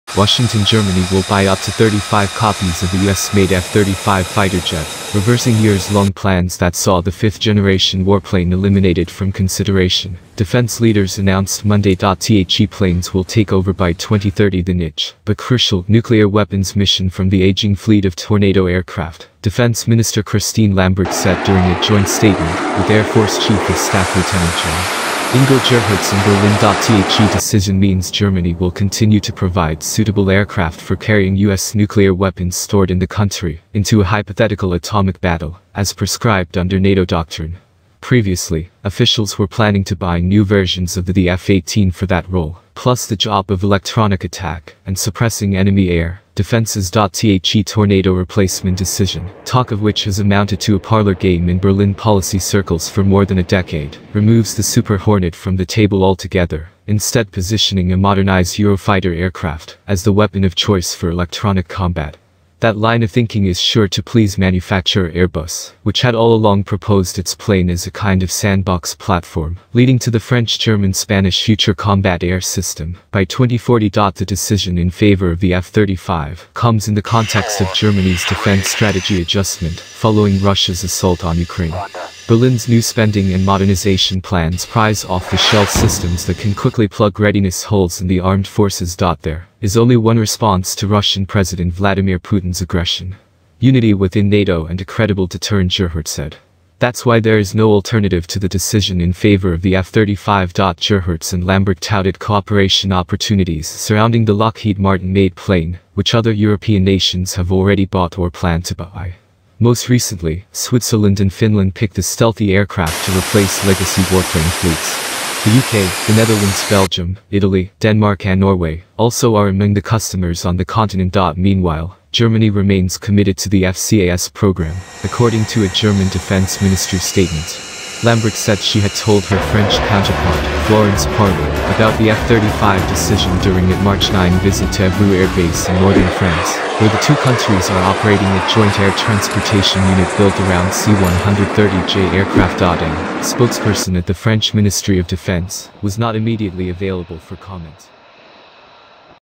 FOR NUCLEAR PREVENTION, GERMANY BUYS UP TO 35 UNITS F-35 JET FROM U.S
Germany will buy up to 35 copies of the U.S.-made F-35 fighter jet, reversing years-long plans that saw the fifth-generation warplane eliminated from consideration, defense leaders announced Monday.

The planes will take over by 2030 the niche, but crucial, nuclear-weapons mission from the aging fleet of Tornado aircraft, Defense Minister Christine Lambrecht said during a joint statement with Air Force Chief of Staff Lt. Gen. Ingo Gerhartz in Berlin.

The decision means Germany will continue to provide suitable aircraft for carrying U.S. nuclear weapons stored in the country into a hypothetical atomic battle, as prescribed under NATO doctrine. Previously, officials were planning to buy new versions of the the F-18 for that role plus the job of electronic attack and suppressing enemy air defenses.

The Tornado-replacement decision, talk of which has amounted to a parlor game in Berlin policy circles for more than a decade, removes the Super Hornet from the table altogether, instead positioning a modernized Eurofighter aircraft as the weapon of choice for electronic combat. That line of thinking is sure to please manufacturer Airbus, which had all along proposed its plane as a kind of sandbox platform leading to the French-German-Spanish Future Combat Air System by 2040.

The decision in favor of the F-35 comes in the context of Germany’s defense strategy adjustment following Russia’s assault on Ukraine. Berlin’s new spending and modernization plans prize off-the-shelf systems that can quickly plug readiness holes in the armed forces.

“There is only one response to [Russian President Vladimir] Putin’s aggression: unity within NATO and a credible deterrent,” Gerhartz said. “That’s why there is no alternative to the decision in favor of the F-35.”

Gerhartz and Lambrecht touted cooperation opportunities surrounding the Lockheed Martin-made plane, which other European nations have already bought or plan to buy. Most recently, Switzerland and Finland picked the stealthy aircraft to replace legacy warplane fleets. The U.K., the Netherlands, Belgium, Italy, Denmark and Norway also are among the customers on the continent.

Meanwhile, Germany remains committed to the FCAS program, according to a German Defense Ministry statement. Lambrecht said she had told her French counterpart, Florence Parly, about the F-35 decision during at March 9 visit to Evreux Air Base in northern France, where the two countries are operating a joint air-transportation unit built around C-130J aircraft.

A spokesperson at the French Ministry of Defence was not immediately available for comment.